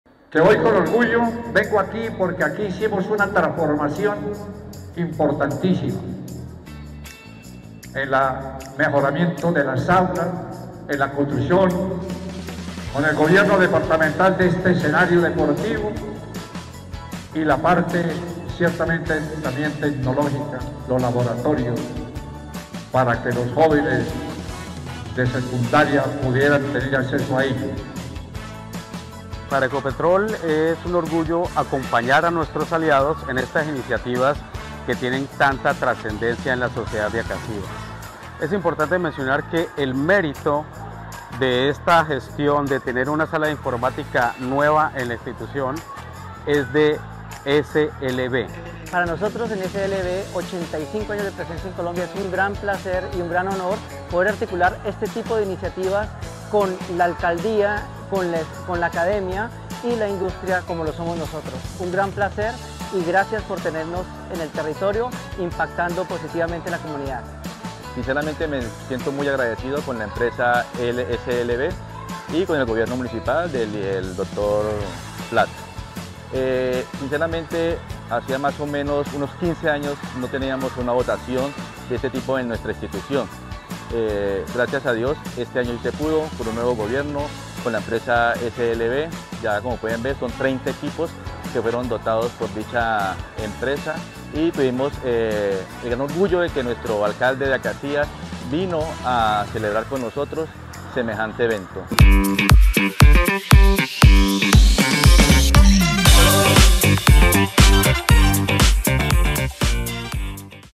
Entrega sala computo Institución Educativa 20 de Julio
Educación 👩🏫| La empresa SLB contratista de Ecopetrol, realizó la entrega de equipos de informática a la Institución Educativa 20 de Julio. 💻

El coronel Carlos Plata, alcalde de Acacías, acompañó a los estudiantes en la entrega de los elementos para la nueva sala de computo.

Seguimos recuperando Acacías con la transformación digital.